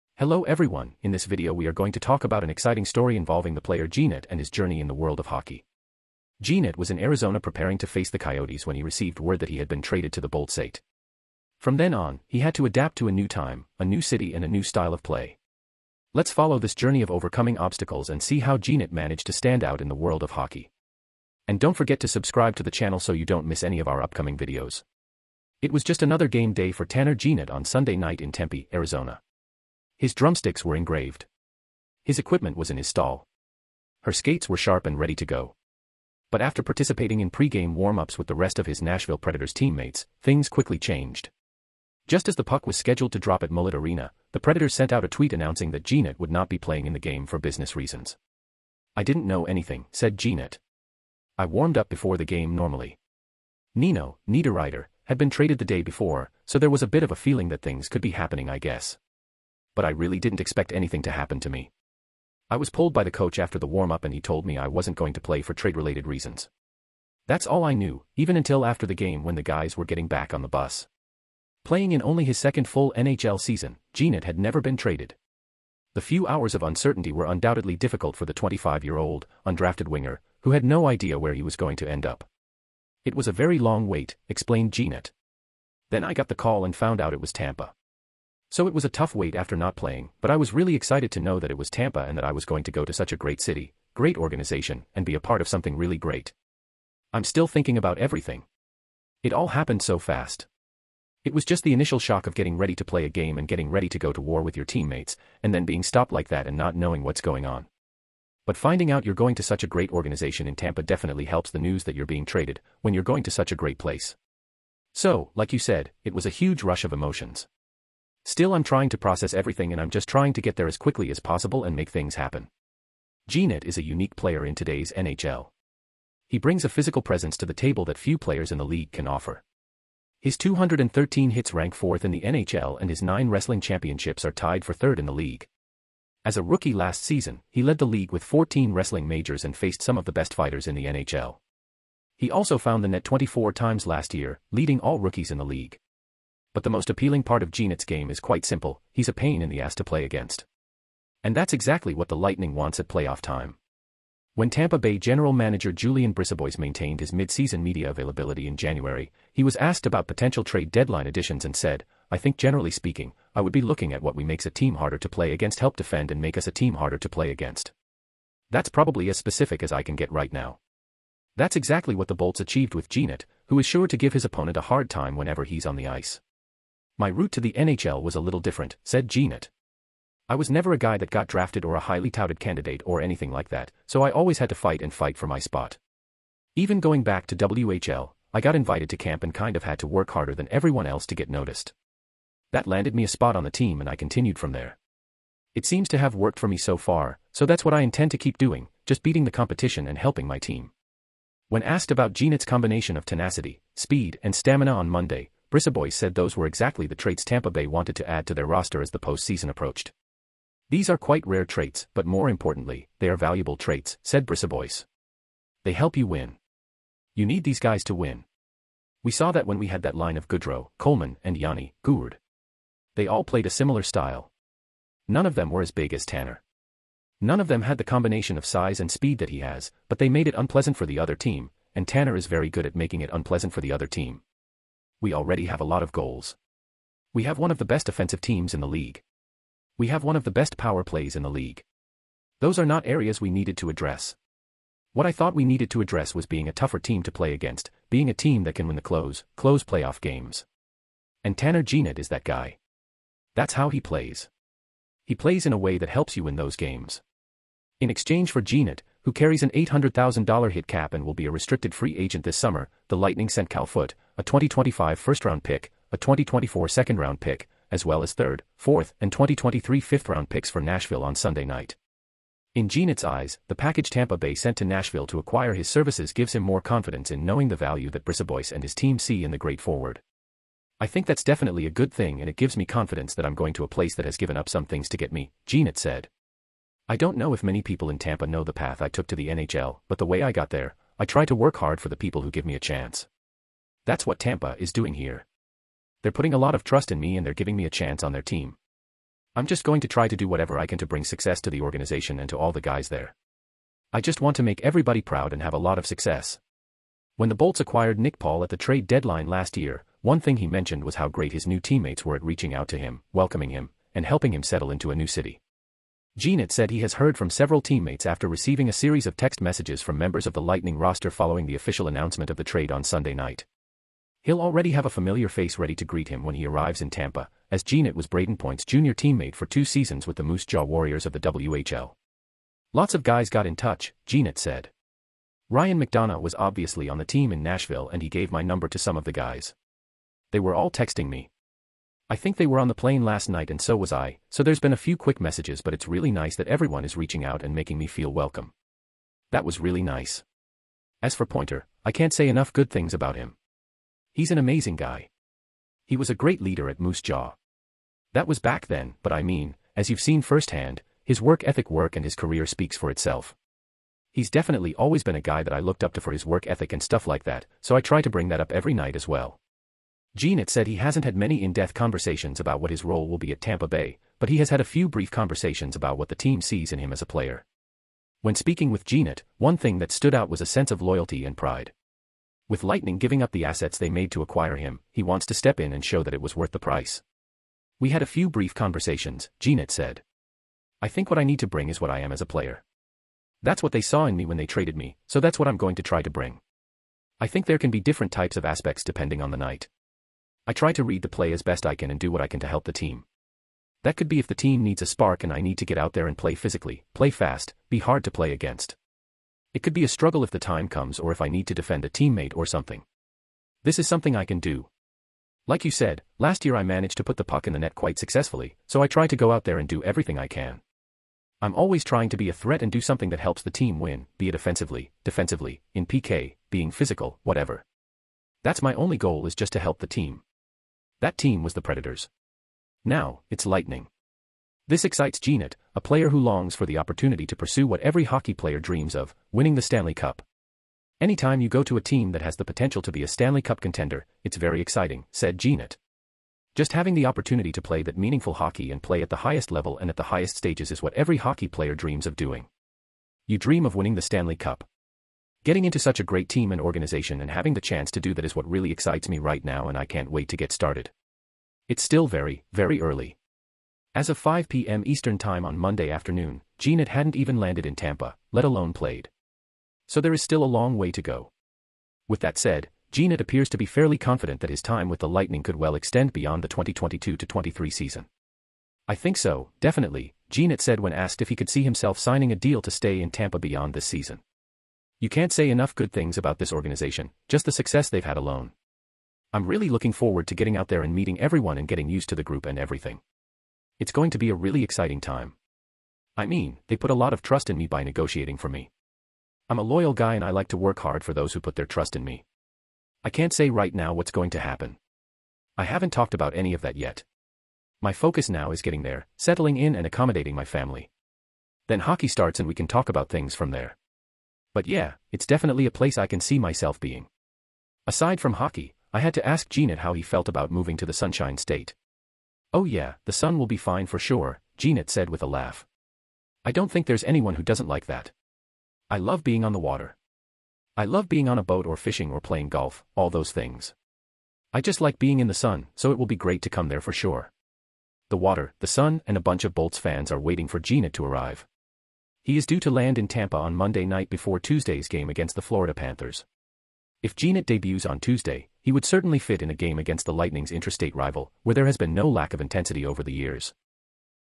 (REVEALED EVERYTHING) I WAS GETTING READY TO FIGHT THE COYOTES WHEN..
Hello everyone, in this video we are going to talk about an exciting story involving the player Jeannot and his journey in the world of hockey. Jeannot was in Arizona preparing to face the Coyotes when he received word that he had been traded to the Boltseit. From then on, he had to adapt to a new time, a new city and a new style of play. Let's follow this journey of overcoming obstacles and see how Jeannot managed to stand out in the world of hockey.

It was just another game day for Tanner Jeannot on Sunday night in Tempe, Arizona. His drumsticks were engraved. His equipment was in his stall. Her skates were sharp and ready to go.

But after participating in pre-game warm-ups with the rest of his Nashville Predators teammates, things quickly changed.

"I didn't know anything," said Jeannot. "I warmed up before the game normally. Nino (Niederreiter) had been traded the day before, so there was a bit of a feeling that things could be happening I guess. But I really didn't expect anything to happen to me.

"I was pulled by the coach after the warm-up and he told me I wasn't going to play for trade-related reasons. That's all I knew, even until after the game when the guys were getting back on the bus."

Playing in only his second full NHL season, Jeannot had never been traded. The few hours of uncertainty were undoubtedly difficult for the 25-year-old, undrafted winger, who had no idea where he was going to end up.

"It was a very long wait," explained Jeannot. "Then I got the call and found out it was Tampa. So it was a tough wait after not playing, but I was really excited to know that it was Tampa and that I was going to go to such a great city, great organization and be a part of something really great.

"I'm still thinking about everything. It all happened so fast. It was just the initial shock of getting ready to play a game and getting ready to go to war with your teammates, and then being stopped like that and not knowing what's going on. .

"But finding out you're going to such a great organization in Tampa definitely helps the news that you're being traded, when you're going to such a great place. So, like you said, it was a huge rush of emotions. Still I'm trying to process everything and I'm just trying to get there as quickly as possible and make things happen."

Jeannot is a unique player in today's NHL. He brings a physical presence to the table that few players in the league can offer. His 213 hits rank fourth in the NHL and his nine wrestling championships are tied for third in the league. As a rookie last season, he led the league with 14 wrestling majors and faced some of the best fighters in the NHL. He also found the net 24 times last year, leading all rookies in the league.

But the most appealing part of Jeannot's game is quite simple - he's a pain in the ass to play against. And that's exactly what the Lightning wants at playoff time.

When Tampa Bay General Manager Julien BriseBois maintained his midseason media availability in January, he was asked about potential trade deadline additions and said, "I think generally speaking, I would be looking at what we makes a team harder to play against Help defend and make us a team harder to play against.

"That's probably as specific as I can get right now."

That's exactly what the Bolts achieved with Jeannot, who is sure to give his opponent a hard time whenever he's on the ice.

"My route to the NHL was a little different," said Jeannot. “I was never a guy that got drafted or a highly touted candidate or anything like that, so I always had to fight and fight for my spot.

“Even going back to WHL, I got invited to camp and kind of had to work harder than everyone else to get noticed. That landed me a spot on the team and I continued from there.

"It seems to have worked for me so far, so that's what I intend to keep doing - just beating the competition and helping my team."

When asked about Jeannot's combination of tenacity, speed and stamina on Monday, BriseBois said those were exactly the traits Tampa Bay wanted to add to their roster as the postseason approached.

“These are quite rare traits, but more importantly, they are valuable traits,” said BriseBois. "They help you win. You need these guys to win.

"We saw that when we had that line of Goodrow, Coleman and Yanni (Gourde). They all played a similar style.

"None of them were as big as Tanner. None of them had the combination of size and speed that he has, but they made it unpleasant for the other team, and Tanner is very good at making it unpleasant for the other team.

"We already have a lot of goals. We have one of the best offensive teams in the league. We have one of the best power plays in the league...